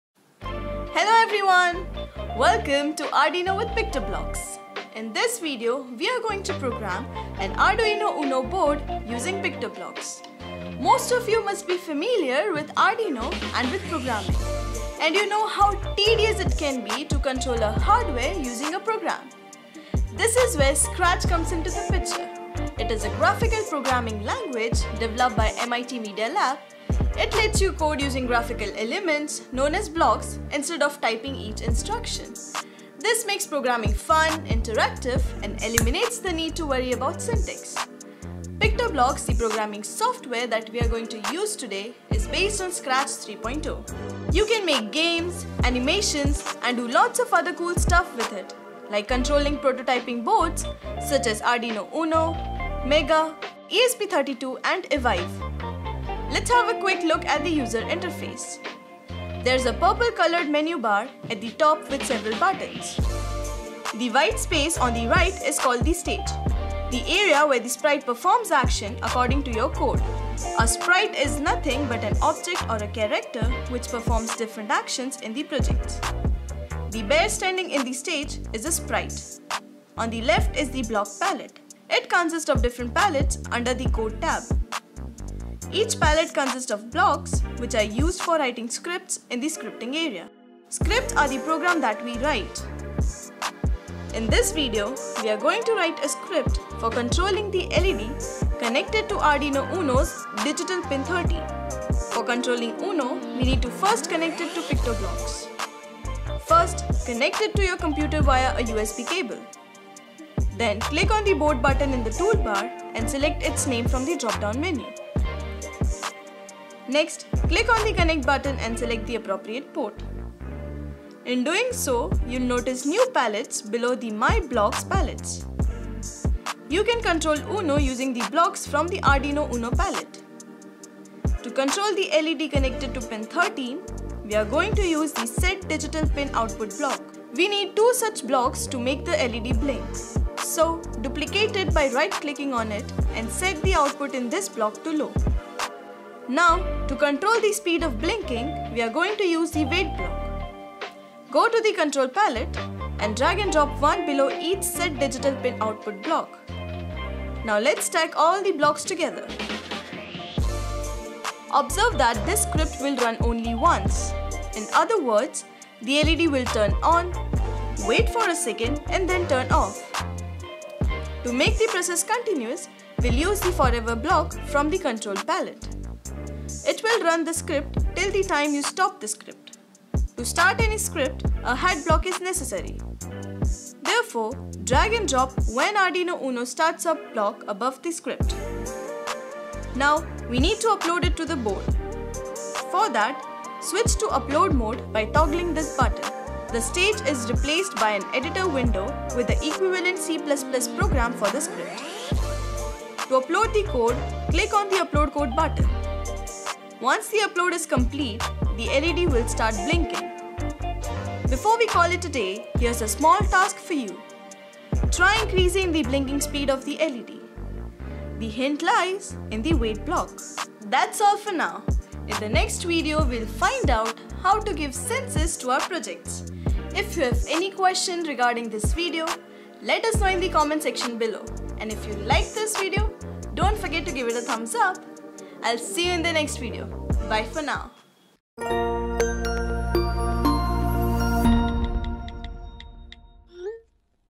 How to Use PictoBlox to Program an Arduino Board (Scratch Based Programming Software)
Learn how to use PictoBlox, a flexible Scratch-like graphical programming software with increased interaction features, to program an Arduino Uno board. Jennifer will first introduce you to PictoBlox and walk you through the interface to offer you a virtual hands-on experience.

PictoBlox is graphical programming software that makes coding fun and easy. It is based on the current version of Scratch. Students may build games and animations, as well as interactive IoT projects, robot programming, and more. PictoBlox also assists kids in the development of critical 21st-century abilities including creativity, logical thinking, and problem-solving.